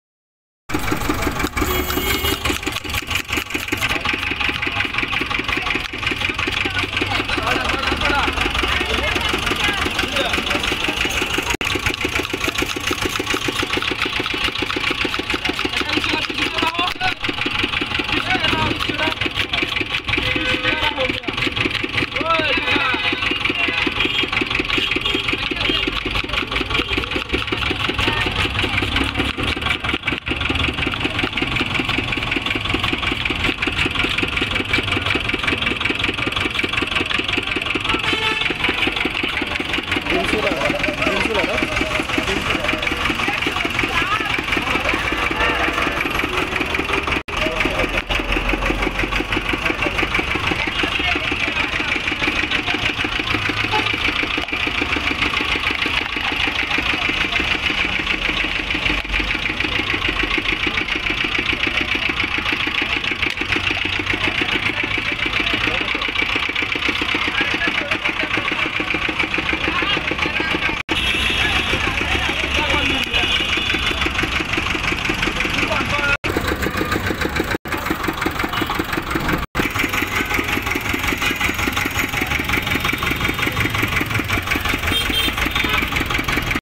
Instant Chola Chaat King I Asian Street Food
Spicy chola chaat is a very healthy food more than other foods.Bangladeshi people enjoy this food very much.it is very famous in Bangladesh, India, Kolkata, & many other South Asian countries.